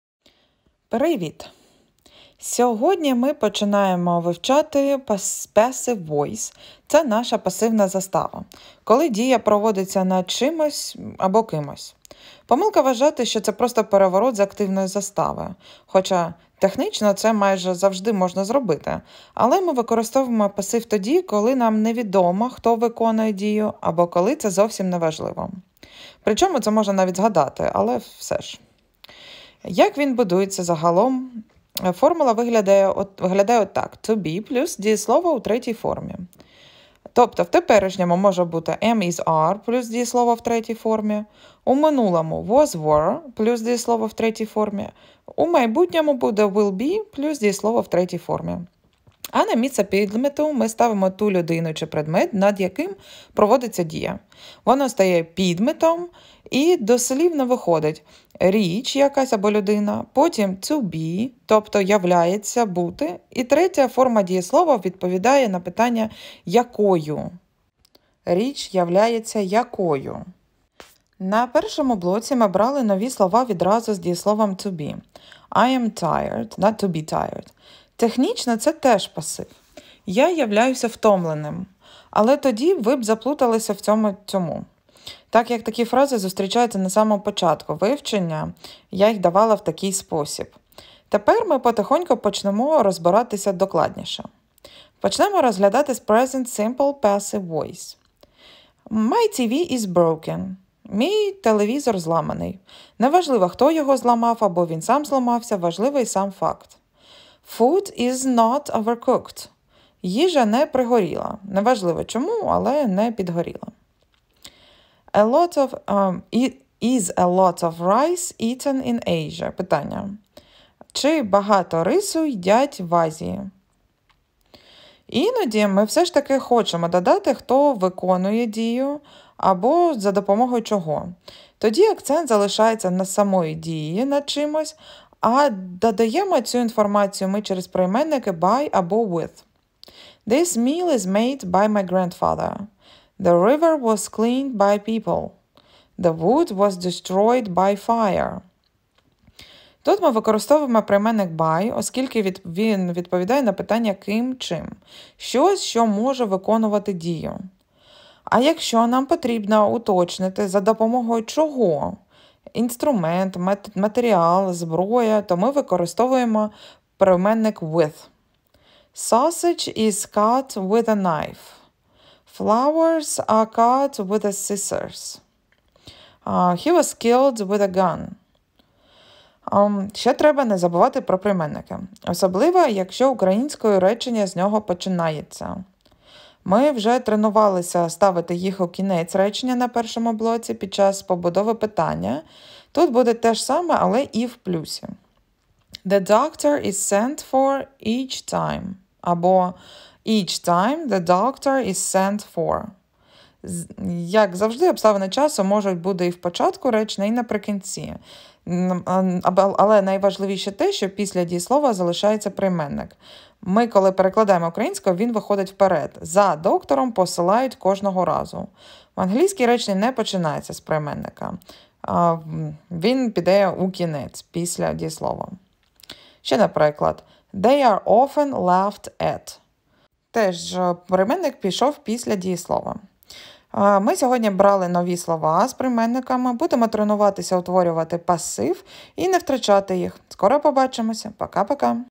Passive Voice в англійській мові: як правильно утворювати та використовувати
Привіт! У цьому відео я проведу вас через процес утворення Passive Voice в англійській мові. Ми розглянемо, як використовувати пасивний стан у різних часах, і детально розберемо основні принципи його застосування.

💡 Що вас чекає у відео?

Просте пояснення правил утворення Passive Voice
Розбір різних часів та їх застосування
🌟 Це відео підходить для:

Початківців та тих, хто хоче глибше зрозуміти англійську граматику
Усі, хто прагне впевнено використовувати Passive Voice у своїй мові
📌 Поглиблюйте свої знання англійської! Завітайте на мій сайт, щоб знайти більше корисних матеріалів та отримати доступ до інтерактивних уроків.